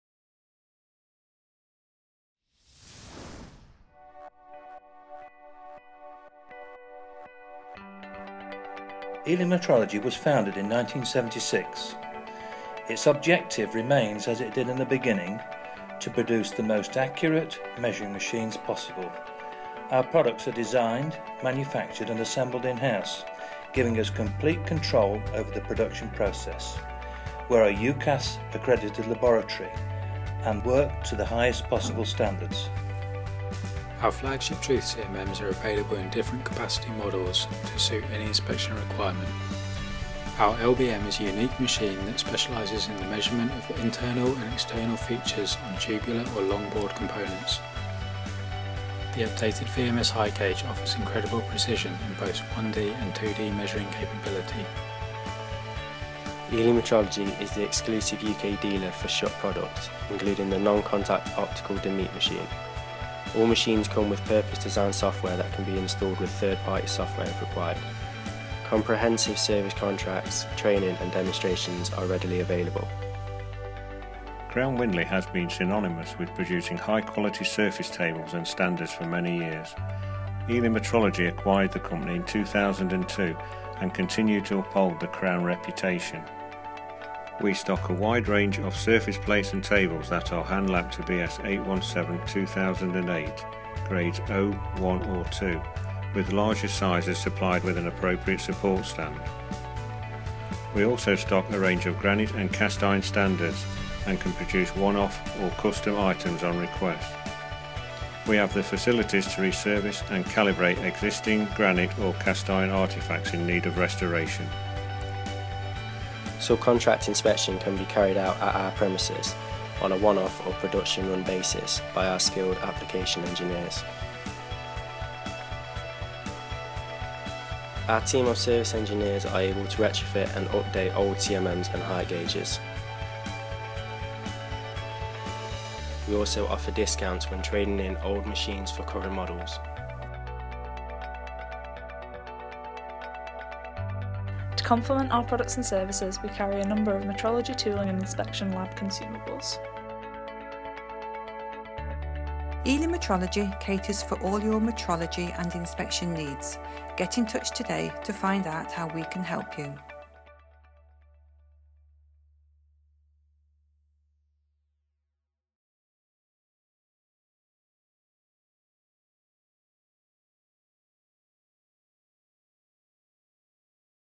Eley Metrology - Products & Services
For over 40 years Eley Metrology Limited has been at the heart of the design and manufacture of high-quality metrology equipment and the precision measurement industry. A comprehensive product line allows for a complete solution to your measurement problems. Our services are covered by our skilled and friendly engineers that are always on-hand to help ensure you have a fantastic experience with us and are supported by our after sales and technical teams.

With products ranging from small metrology tooling items to full coordinate measuring machine services and inspection, we have your company's measurement needs covered. Our products are designed and produced to the highest manufacturing quality level and are supported by our UKAS Laboratory (no. 0333).

Eley Metrology caters for industries such as aerospace, automotive, oil and gas and appliance making. Our clients range from large manufacturing companies to inspection laboratories for quality control.